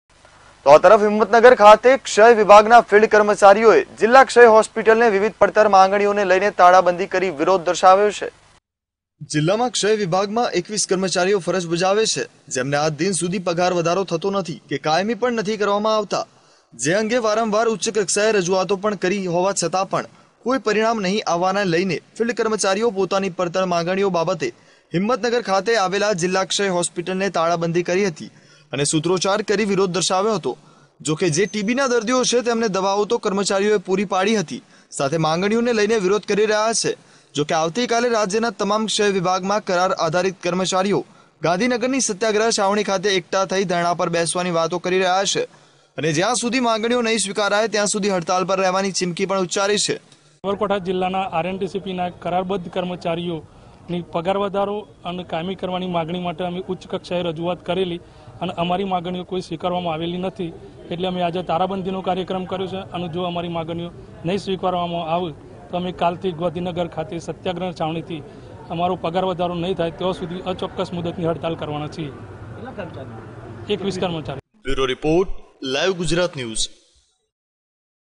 HIMATNAGAR MA KSHAY VIBHAG NA FIELD KARMACHARIO E TALABANDHI KARI VIRODH KARYO
જીલ્લા ક્ષય વિભાગમાં 21 કમર્ચારીઓ ફરજ બજાવે છે જેમને આજદિન સુધી પગાર વધારો થતો નથી કે કાયમી પણ નથી કરવામાં આવતા જે અંગે વારંવાર ઉચ્ય કક્ષાએ રજુઆતો પણ કરી છતાં કોઈ પરિણામ નહિ આવવાને લઈને ફિલ્ડ કર્મચારીઓ પોતાની પડતર માંગણીઓને લઈને હિમતનગર ખાતે આવેલ જીલ્લા ક્ષય હોસ્પિટલને તાળાબંધી કરી હતી અને સુત્રોચ્ચાર કરી વિરોધ દર્શાવ્યો હતો.જોકે જે ટીબી ના દર્દીઓ છે તેમને દવાઓ તો કર્મચારીઓએ પૂરી પડી દીધેલ છે સાથે માંગણીઓને લઈને વિરોધ કરી રહ્યા છે.જેથી આવતી કાલે રાજ્યના તમામ ક્ષય વિભાગમાં કરાર આધારિત કર્મચારીઓ ગાંધીનગર ની સત્યાગ્રહ છાવણી ખાતે એકઠા થઇ ધરણા પર બેસવાની વાત કરી છે જ્યાં સુધી માંગણીઓ નહિ સ્વીકારાય ત્યાં સુધી હડતાલ પર રહેવાની ચીમકી આપી છે